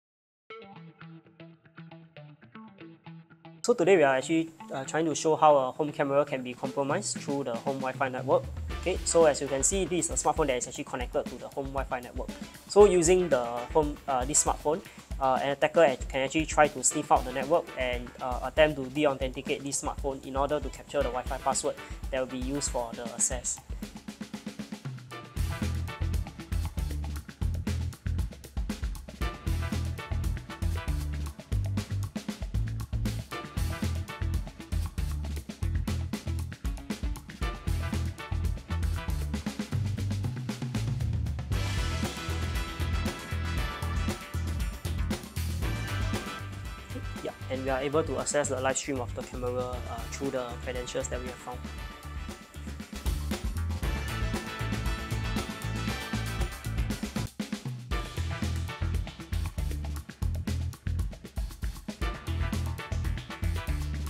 Hacking Demo of an IP camera
Having a weak Wi-Fi password can lead to some serious consequences! Watch this hacking demonstration where officers from CSA's Cybersecurity Engineering Centre show how a weak Wi-Fi password could compromise a connected IP camera.